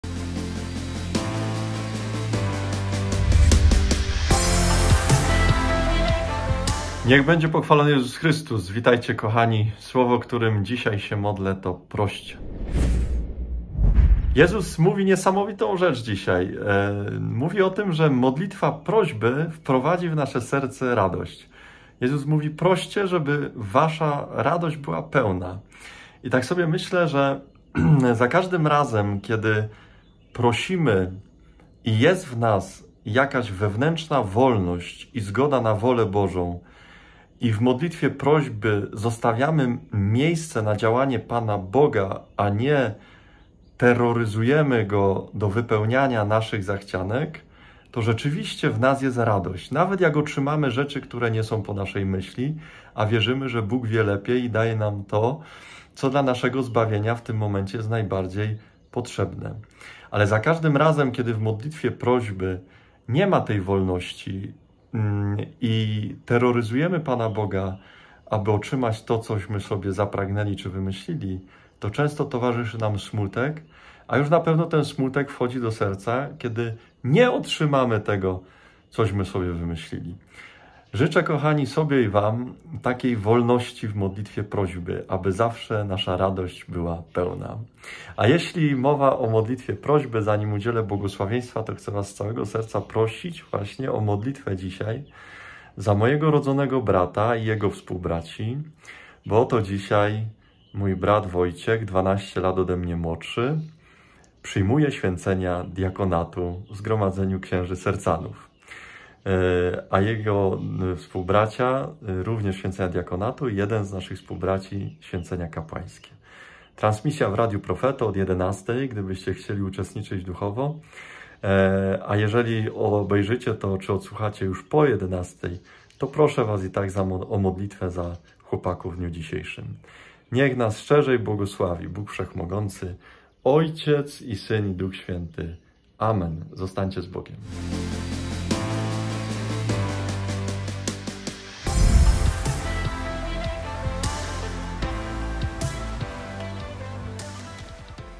Ks. Michał Olszewski SCJ - VLOG JEDNO SŁOWO - Proście
"O cokolwiek prosilibyście Ojca, da wam w imię moje. Do tej pory o nic nie prosiliście w imię moje: proście, a otrzymacie, aby radość wasza była pełna." J 16,23b-28